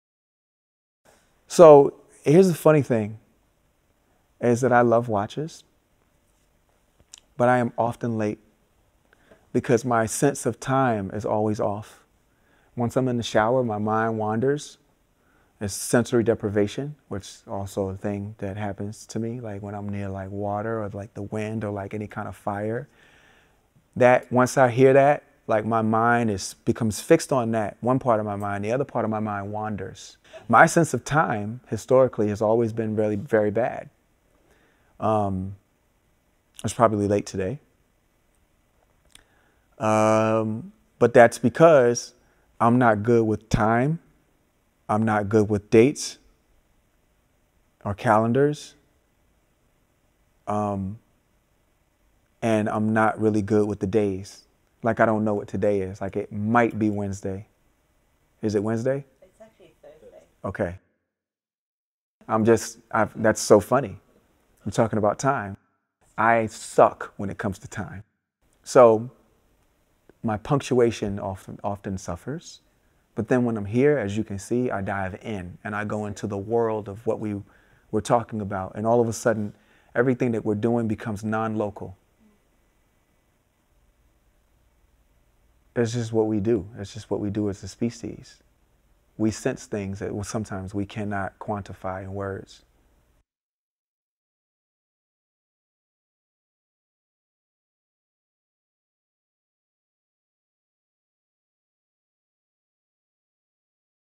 Pharrell Williams X RICHARD MILLE – TIME
‘What Richard does is build something that lets you take the metric of what time looks like. That's a wizard-level art form.’ Exclusive interview of Pharrell Williams about his passion for watchmaking and his perception of time.

WeChat: RICHARDMILLE里查德米尔